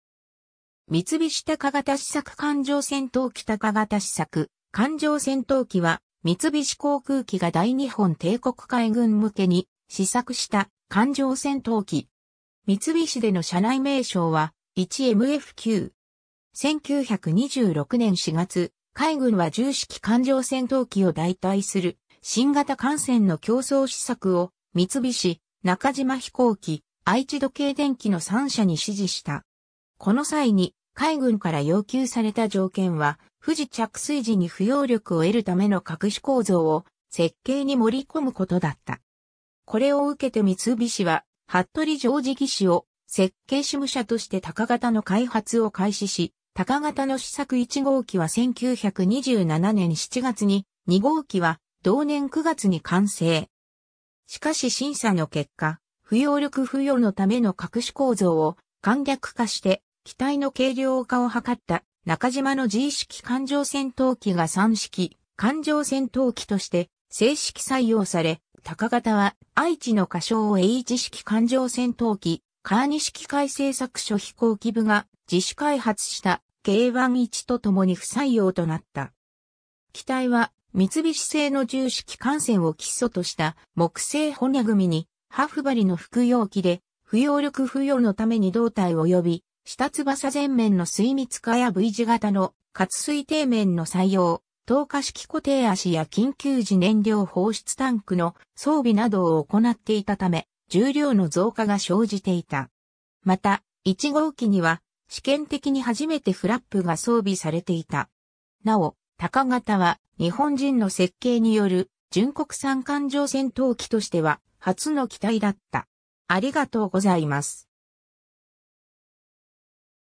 鷹型試作艦上戦闘機
三菱 鷹型試作艦上戦闘機 鷹型試作艦上戦闘機（たかがたしさくかんじょうせんとうき）は、三菱航空機が大日本帝国海軍向けに試作した艦上戦闘機。
三菱での社内名称は「1MF9」。
 1926年（大正15年）4月、海軍は一〇式艦上戦闘機を代替する新型艦戦の競争試作を三菱、中島飛行機、愛知時計電機の三社に指示した。
この際に海軍から要求された条件は、不時着水時に浮揚力を得るための各種構造を設計に盛り込むことだった。
これを受けて三菱は服部譲次技師を設計主務者として鷹型の開発を開始し、鷹型の試作一号機は1927年（昭和2年）7月に、二号機は同年9月に完成。
しかし審査の結果、浮揚力付与のための各種構造を簡略化して機体の軽量化を計った中島のG式艦上戦闘機が三式艦上戦闘機として制式採用され、鷹型は愛知の仮称H式艦上戦闘機、川西機械製作所飛行機部が自主開発したK-11とともに不採用となった。
 機体は三菱製の一〇式艦戦を基礎とした木製骨組に羽布張りの複葉機で、浮揚力付与のために胴体および下翼前面の水密化やV字型の滑水底面の採用、投下式固定脚や緊急時燃料放出タンクの装備などを行っていたため、重量の増加が生じていた。
また、一号機には試験的に初めてフラップが装備されていた。
なお、鷹型は日本人の設計による純国産艦上戦闘機としては初の機体だった。